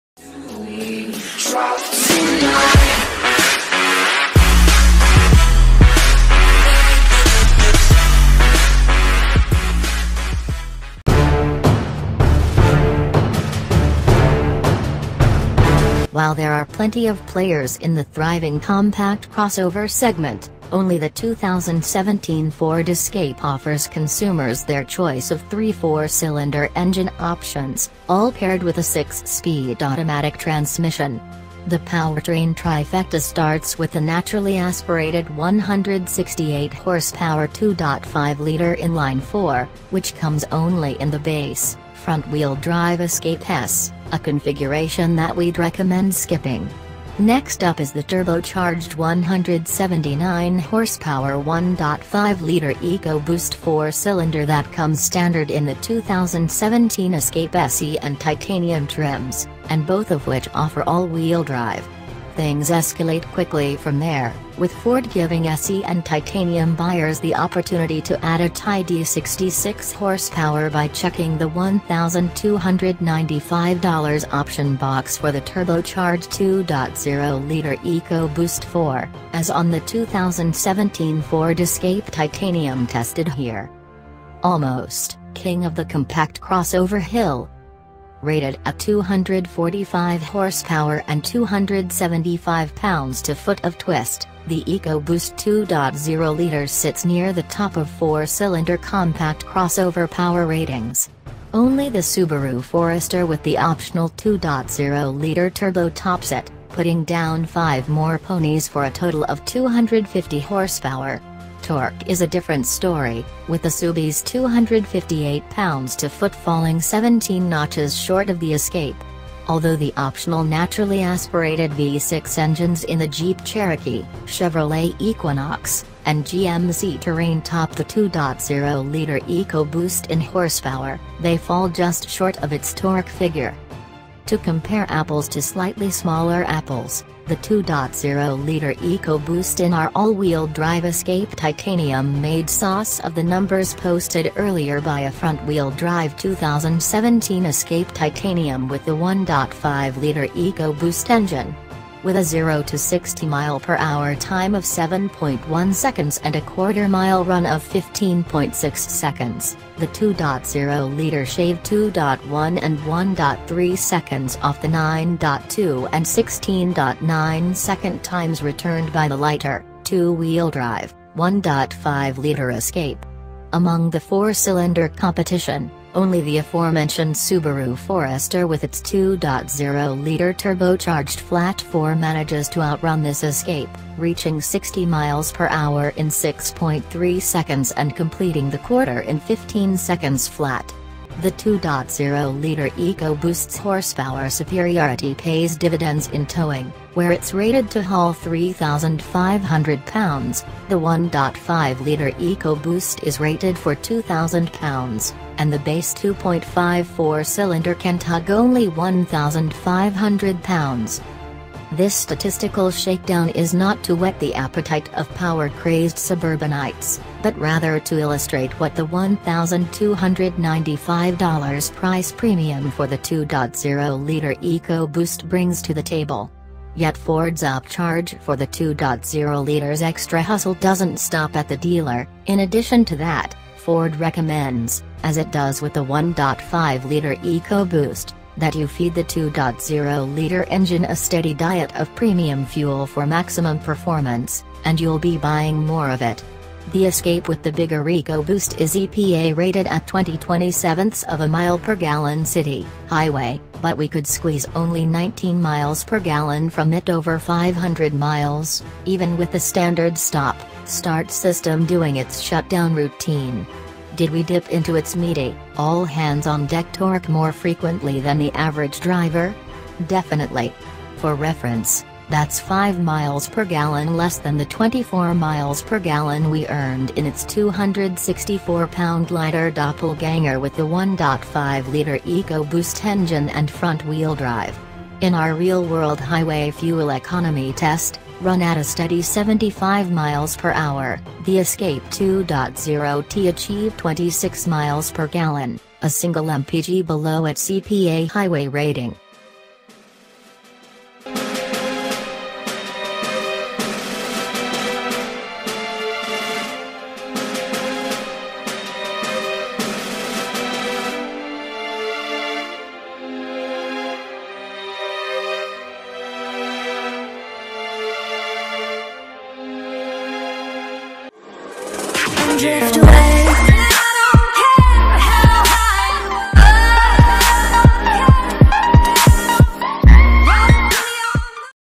2017 Ford Escape 2 0L EcoBoost AWD Review
2017 Ford Escape 2 0L EcoBoost AWD Review. While there are plenty of players in the thriving compact-crossover segment, only the 2017 Ford Escape offers consumers their choice of three four-cylinder engine options, all paired with a six-speed automatic transmission. The powertrain trifecta starts with the naturally aspirated 168-hp 2.5-liter inline-four, which comes only in the base, front-wheel-drive Escape S, a configuration that we’d recommend skipping. Next up is the turbocharged 179-hp 1.5-liter EcoBoost four-cylinder that comes standard in the 2017 Escape SE and Titanium trims, and both of which offer all-wheel drive. Things escalate quickly from there, with Ford giving SE and Titanium buyers the opportunity to add a tidy 66 horsepower by checking the $1295 option box for the turbocharged 2.0-liter EcoBoost four, as on the 2017 Ford Escape Titanium tested here.

(Almost) King of the Compact-Crossover Hill

Rated at 245 horsepower and 275 lb-ft of twist, the EcoBoost 2.0-liter sits near the top of four-cylinder compact-crossover power ratings. Only the Subaru Forester with the optional 2.0-liter turbo tops it, putting down five more ponies for a total of 250 horsepower. Torque is a different story, with the Subie’s 258 lb-ft falling 17 notches short of the Escape. Although the optional naturally aspirated V-6 engines in the Jeep Cherokee, Chevrolet Equinox, and GMC Terrain top the 2.0-liter EcoBoost in horsepower, they fall just short of its torque figure.

To compare apples to slightly smaller apples, the 2.0-liter EcoBoost in our all-wheel-drive Escape Titanium made sauce of the numbers posted earlier by a front-wheel-drive 2017 Escape Titanium with the 1.5-liter EcoBoost engine. With a zero-to-60-mph time of 7.1 seconds and a quarter-mile run of 15.6 seconds, the 2.0-liter shaved 2.1- and 1.3-seconds off the 9.2- and 16.9-second times returned by the lighter, two-wheel-drive, 1.5-liter Escape. Among the four-cylinder competition, only the aforementioned Subaru Forester with its 2.0-liter turbocharged flat-four manages to outrun this Escape, reaching 60 mph in 6.3 seconds and completing the quarter in 15 seconds flat. The 2.0-liter EcoBoost’s horsepower superiority pays dividends in towing, where it’s rated to haul 3500 pounds; the 1.5-liter EcoBoost is rated for 2000 pounds, and the base 2.5 four-cylinder can tug only 1500 pounds.

This statistical shakedown is not to whet the appetite of power-crazed suburbanites, but rather to illustrate what the $1295 price premium for the 2.0-liter EcoBoost brings to the table. Yet Ford’s upcharge for the 2.0-liter’s extra hustle doesn’t stop at the dealer; in addition to that, Ford recommends (as it does with the 1.5-liter EcoBoost) that you feed the 2.0-liter engine a steady diet of premium fuel for maximum performance—and you’ll be buying more of it. The Escape with the bigger EcoBoost is EPA rated at 20/27 mpg city/highway, but we could squeeze only 19 mpg from it over 500 miles, even with the standard stop/start system doing its shutdown routine. Did we dip into its meaty, all-hands-on-deck torque more frequently than the average driver? Definitely. For reference, that’s 5 mpg less than the 24 mpg we earned in its 264-pound-lighter doppelgänger with the 1.5-liter EcoBoost engine and front-wheel drive. In our real-world highway fuel-economy test, run at a steady 75 mph, the Escape 2.0T achieved 26 mpg, a single mpg below its EPA highway rating.

With full grunt online at a relatively low 3000 rpm, the power to merge or dart for holes in traffic is never more than a pedal stomp away. Gear swaps are performed by a six-speed automatic with manual shifting capability, the sole transmission available across the lineup. Programmed for smooth performance rather than split-second action, the transmission will likely never give the average buyer a second thought. Only once did we catch it flat-footed and slow to react to an abrupt application of full throttle, where it missed a beat before downshifting. Shifting for yourself is an option (via steering-wheel-mounted paddles on the SE and Titanium and also by a thumb switch on the shift lever), but if compact-class thrills are your ultimate goal, Ford sells a dandy hatch called the Focus ST that will let you get your groove on wherever and whenever you wish.